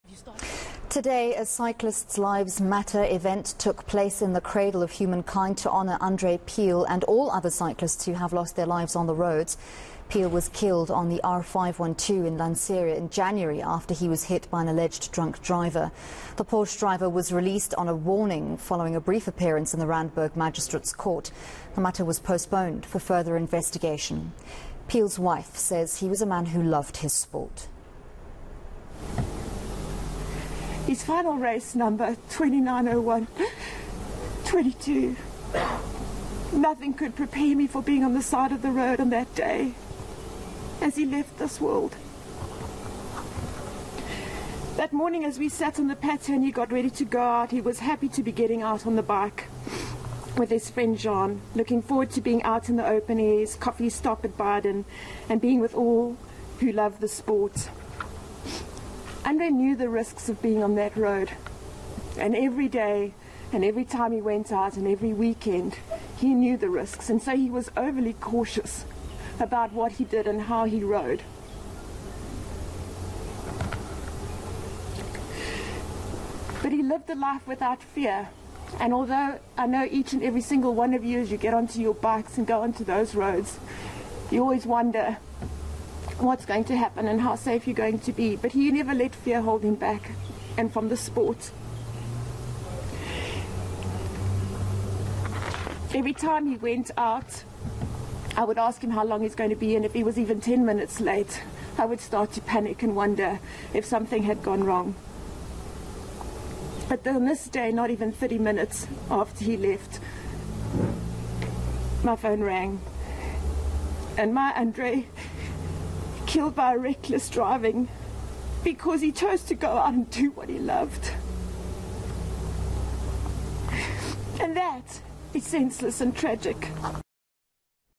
Cycling event honours Piehl and other fallen cyclists
Today, a CyclistsLivesMatter event took place in the Cradle of Humankind to honour André Piehl and all other cyclists who have lost their lives on the roads. Piehl was killed on the R512 in Lanseria in January after he was hit by an alleged drunk driver. Courtesy of DStv403